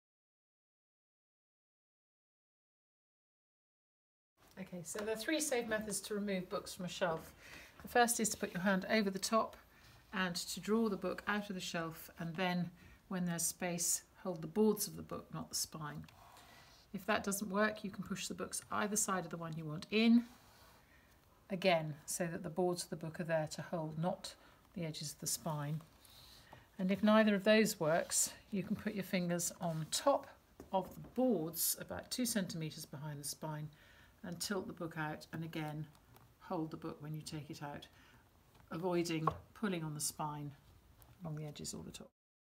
You've probably been taking your books off your bookshelves wrong for years
Caroline Bendix is one of Britain's most experienced and eminent librarians — here, she shows us the proper way to take a book off a shelf without damaging it.
Caroline was filmed by Mark Williamson for Country Life at Houghton House, Little Houghton, Northamptonshire.